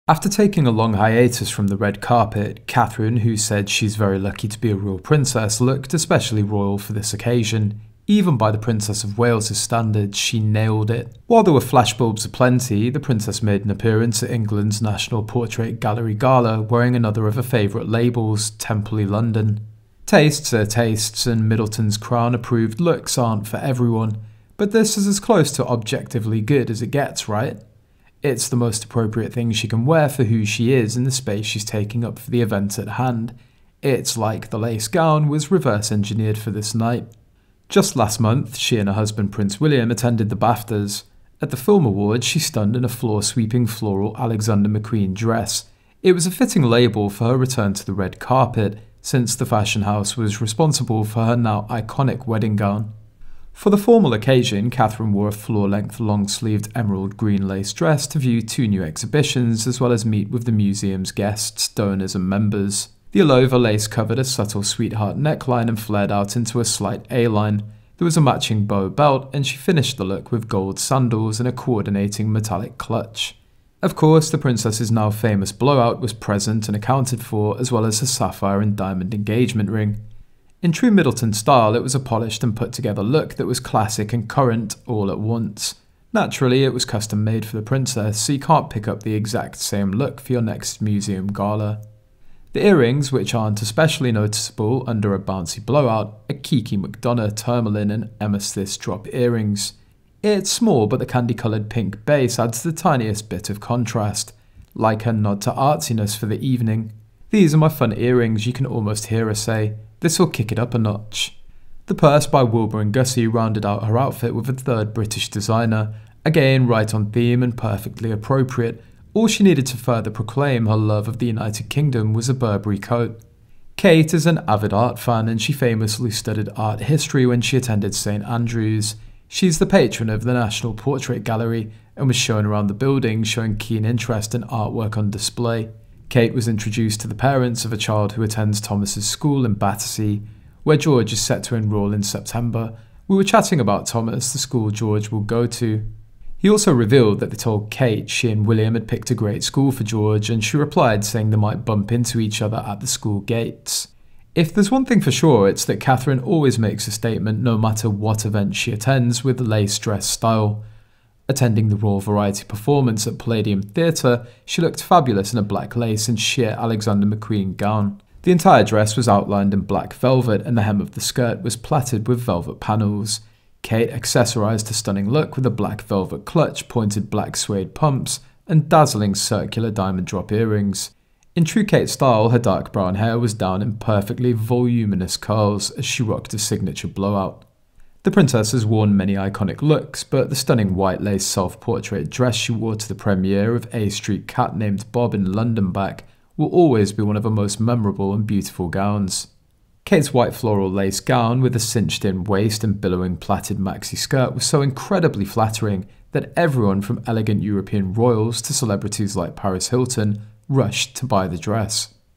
Catherine Dazzles In Gorgeous Green Lace Gown And Stunning Red Ruby Earrings At Portrait Gala
The entire dress was outlined in black velvet and the hem of the skirt was pleated with velvet panels. Kate accessorized her stunning look with a black velvet clutch, pointed black suede pumps, and dazzling circular diamond drop earrings. In true Kate style, her dark brown hair was down in perfectly voluminous curls, as she rocked her signature blowout.